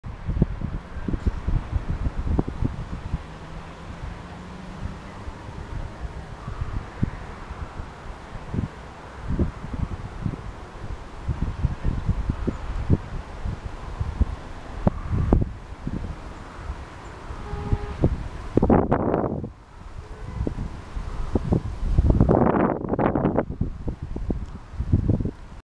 Großbrand in Deutsch-Wagram am 11.10.2011
Massive Rauchentwicklung beim Großbrand der Lagerhalle der Firma OLIO - Spezial Speisefett und Speiseöl GmbH am Dienstag Nachmittag, den 11. Oktober 2011 (Zeit:16:26) in Deutsch-Wagram, Ortsteil Helmahof, Bezirk Gänserndorf in Niederösterreich - "Ecke" Gutenbergstraße / Bockfließerstraße hinter dem Gelände der Firma Wilding KG

jimiivan.at